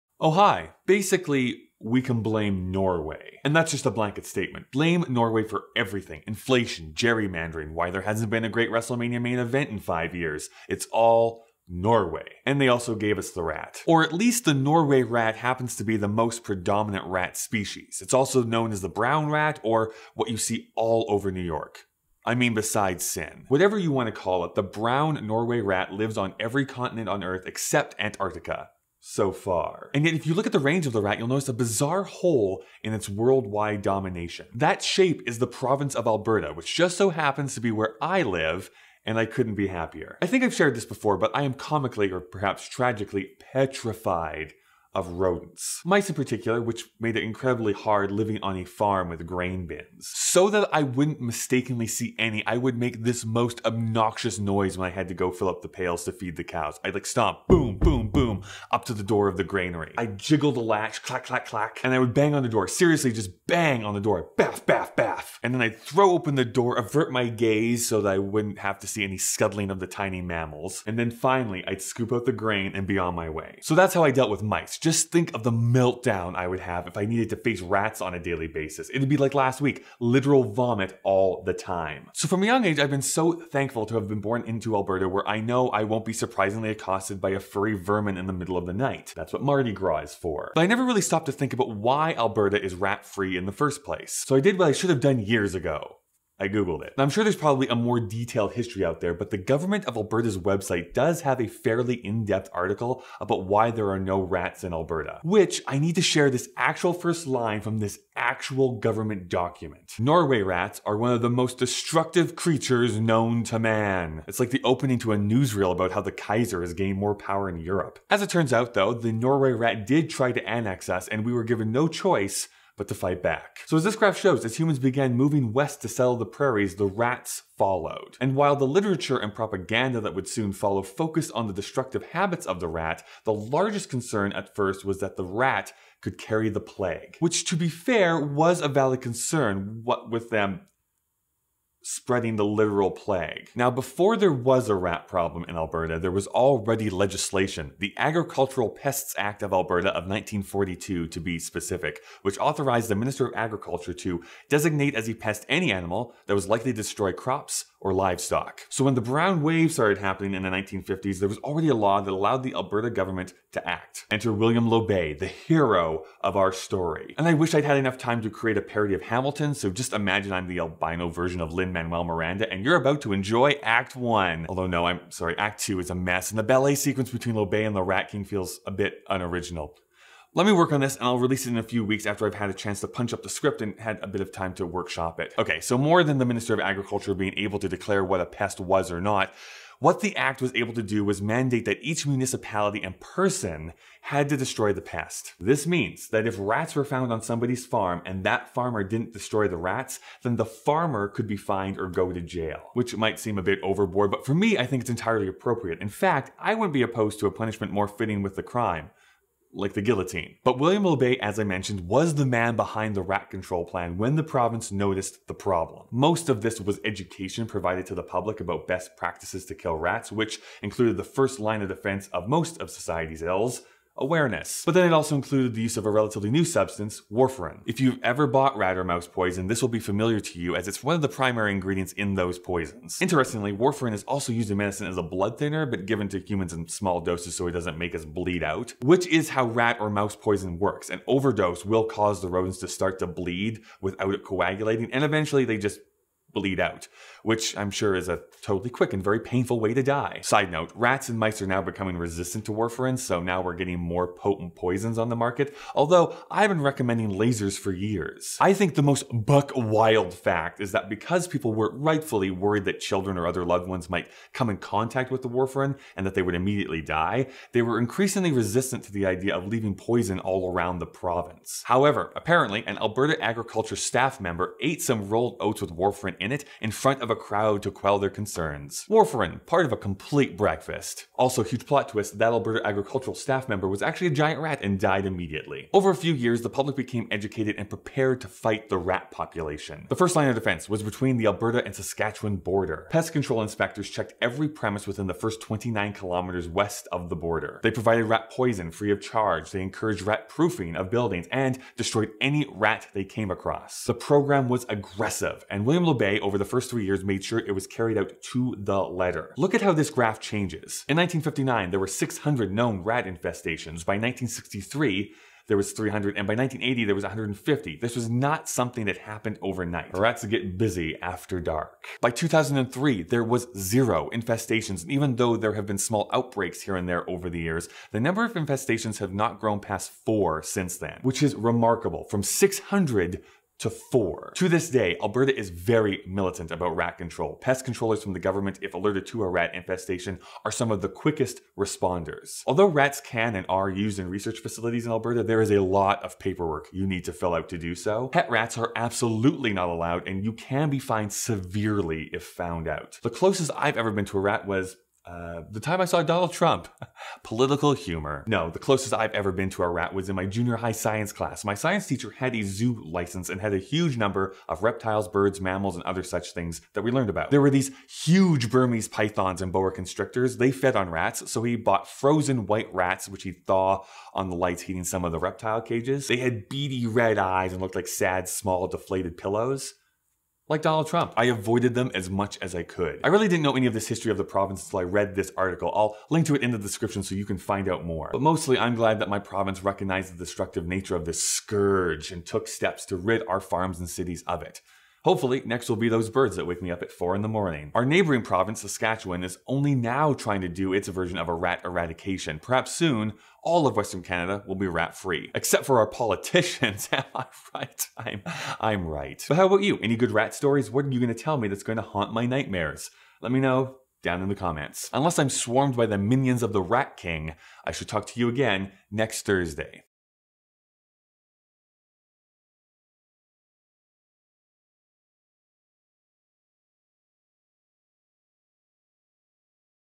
Why There Are No Rats in Alberta
The world freaked out last week over the fact that Alberta has a total of 0 rats! This week I found out exactly why & how that happened.

Government of Alberta document about why there are no rats